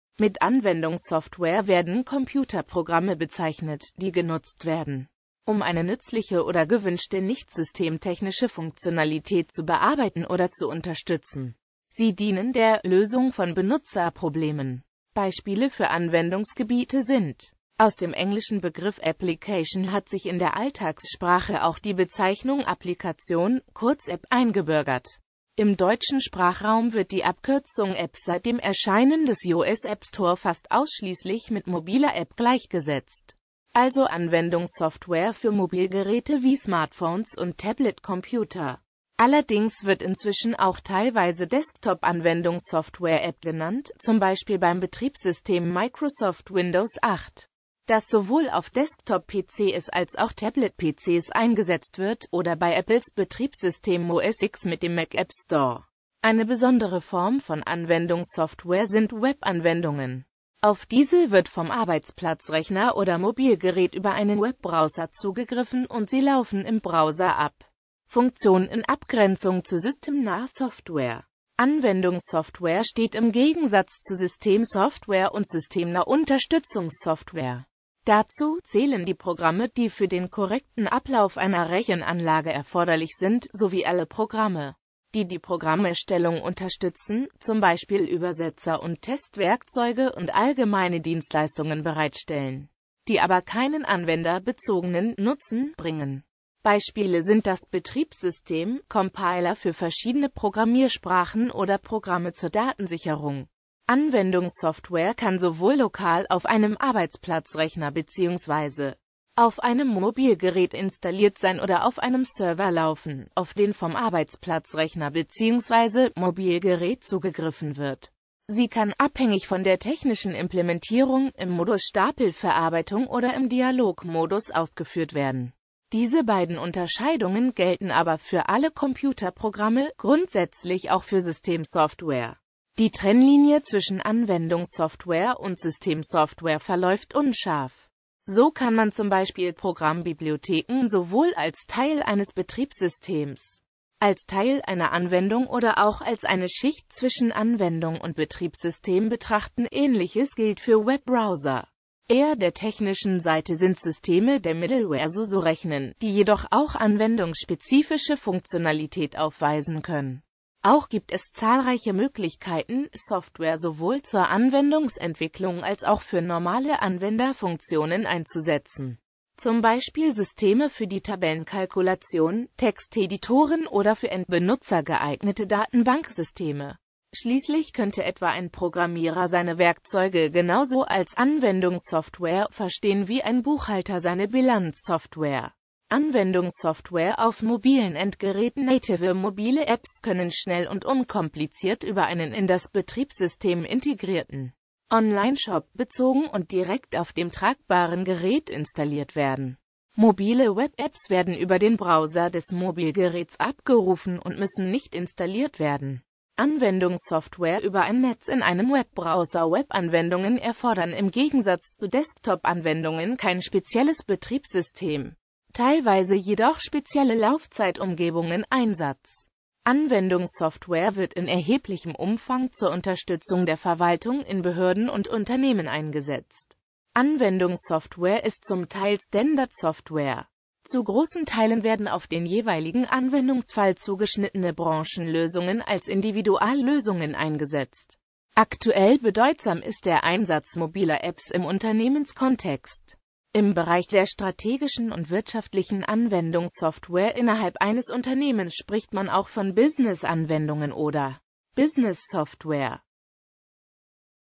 Anwendungssoftware
Vielen Dank für Ihre Unterstützung:

Mit Anwendungssoftware werden Computerprogramme bezeichnet, die genutzt werden, um eine nützliche oder gewünschte nicht systemtechnische Funktionalität zu bearbeiten oder zu unterstützen.Sie dienen der „Lösung von Benutzerproblemen“.Beispiele für Anwendungsgebiete sind: Bildbearbeitung, Videobearbeitung, Textverarbeitung, Tabellenkalkulation, Computerspiele, bestimmte betriebliche Funktionen wie Finanzbuchhaltung, Warenausgang und so weiter,
Aus dem englischen Begriff application hat sich in der Alltagssprache auch die Bezeichnung Applikation, kurz App, eingebürgert.Im deutschen Sprachraum wird die Abkürzung App seit dem Erscheinen des iOS App Store fast ausschließlich mit mobiler App gleichgesetzt, also Anwendungssoftware für Mobilgeräte wie Smartphones und Tabletcomputer.

Bild-Copyright-Informationen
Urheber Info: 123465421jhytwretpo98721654